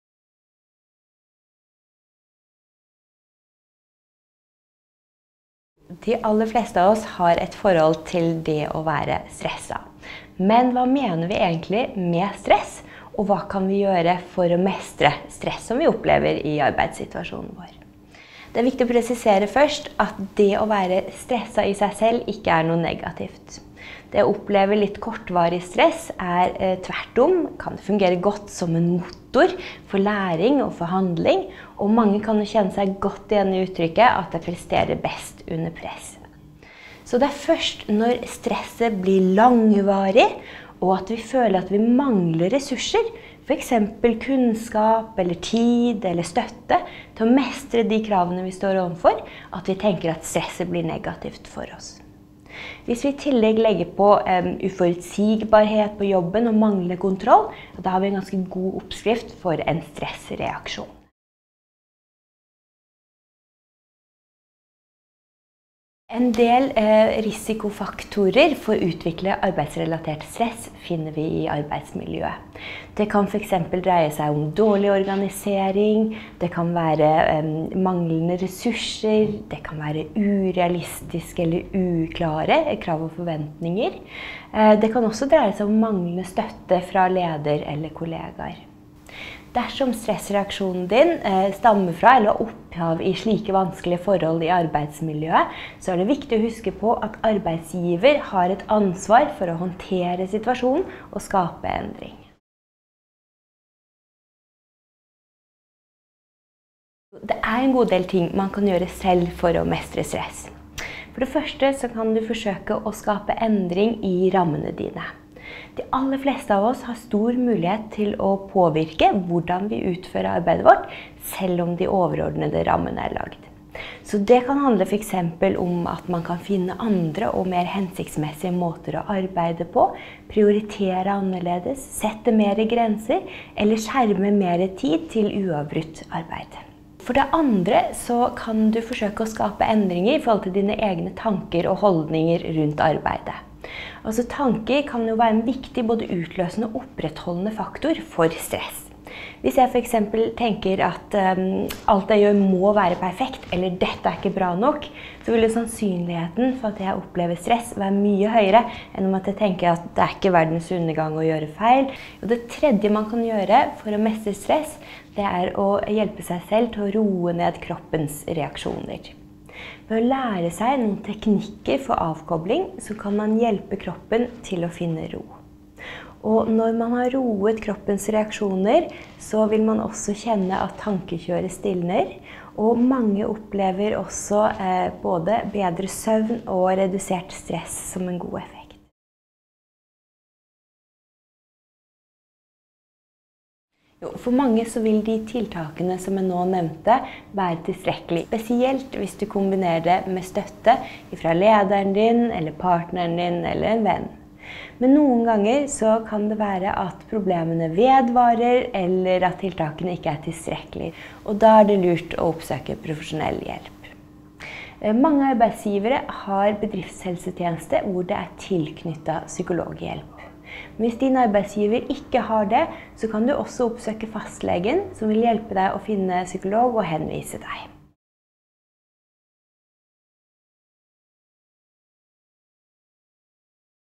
Hvordan mestre stress i arbeidslivet? Psykolog Tove Helene Edvardsen
Hvordan mestre stress i arbeidslivet? Hva kan jeg gjøre selv, hva kan jeg forvente av arbeidsgiver? Psykolog Tove Helene Edvardsen forteller.